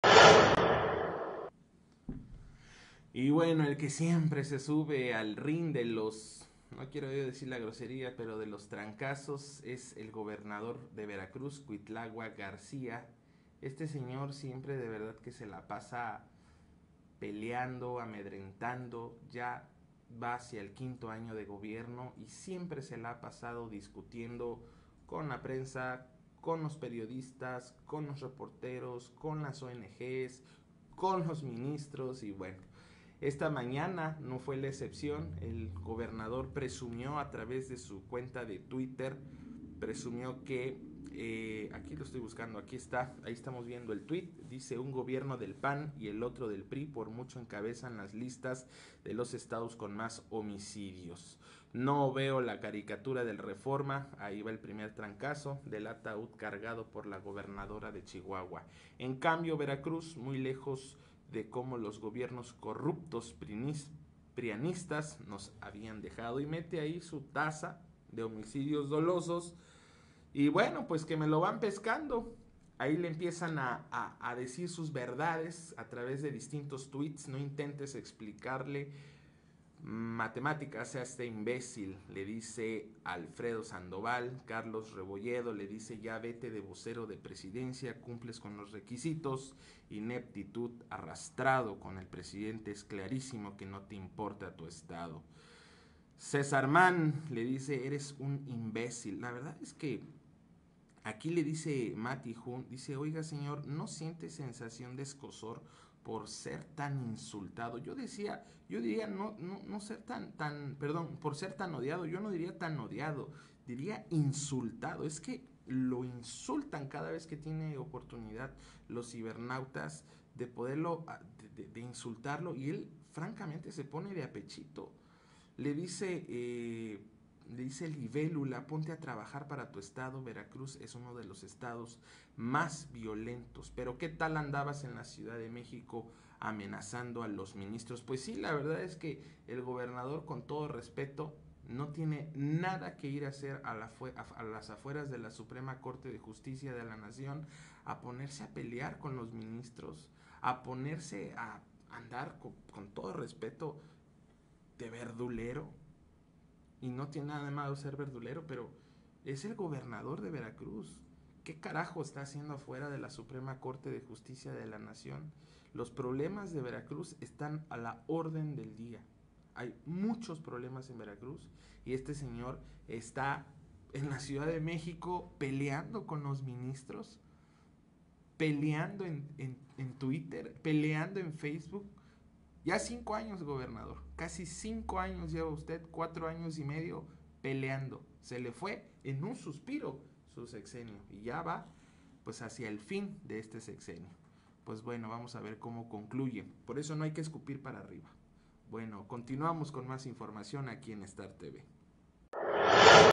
Cuitláhuac García peleando en Twitter ￼
Nuevamente el gobernador Cuitláhuac García gobernador de Veracruz arremete en Twitter contra gobiernos estatales del pasado ￼